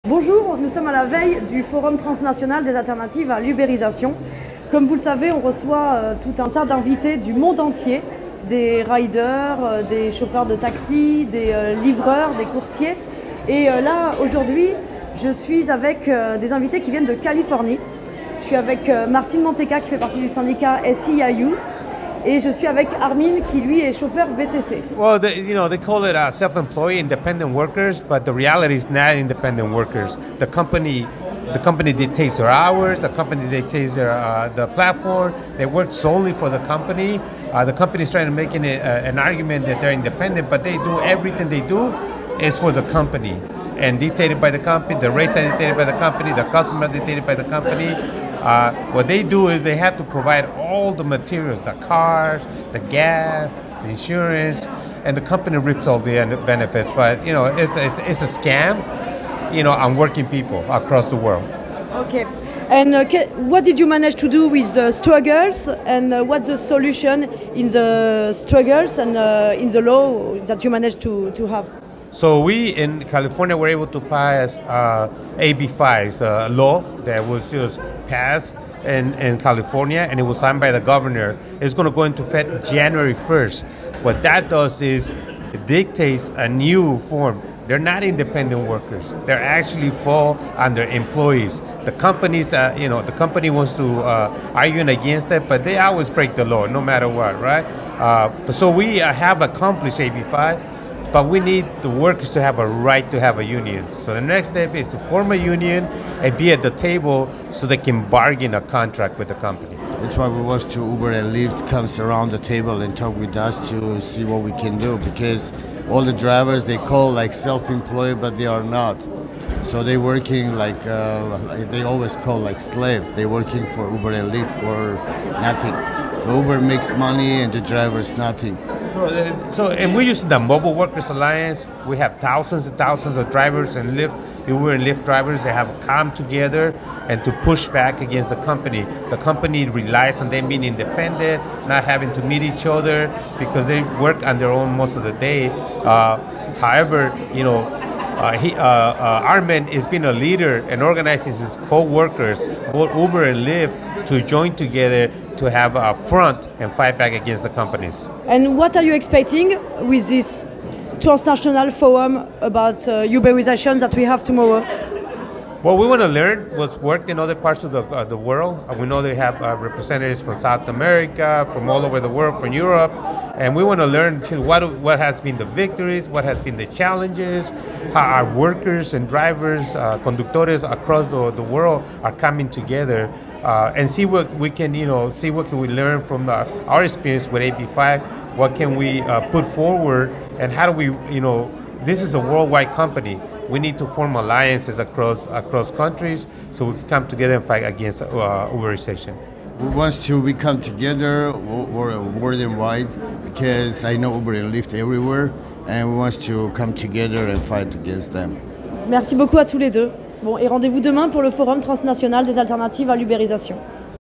Loi AB5, grèves, combats à venir, en Californie, la résistance à l'ubérisation s'organise !
"L'économie des plateformes repose sur le fait qu'ils font croire que les travailleurs sont des indépendants, mais ils ne le sont pas !"

En Californie, la loi AB5 imposera aux plateformes de requalifier les travailleurs ubérisés en salariés dès le 01/01.

Demain au Parlement européen se tiendra le Transnational forum of Alternatives to Uberisation.